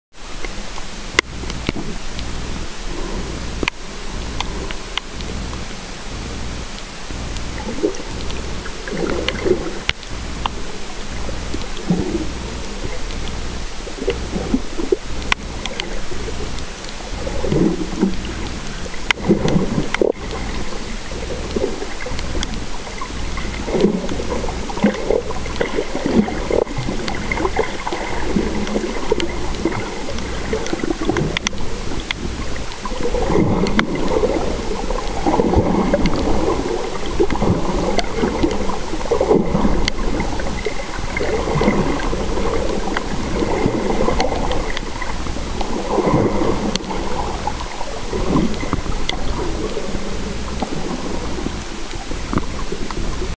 Flying Fish
Snorkeling in Runaway Bay, Jamaica